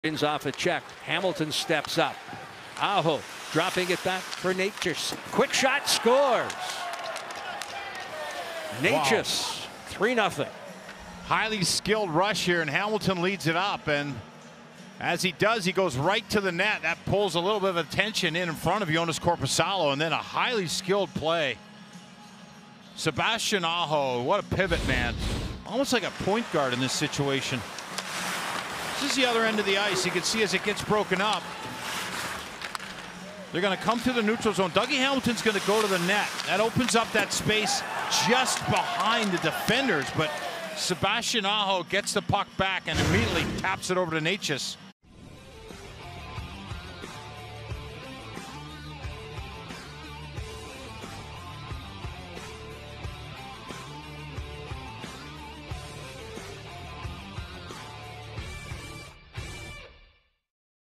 Martin Necas Finishes Off Pretty Hurricanes Passing Play
Sebastian Aho and Martin Necas snap the puck around and Necas puts the finishing touches on the play, buring a shot past Joonas Korpisalo.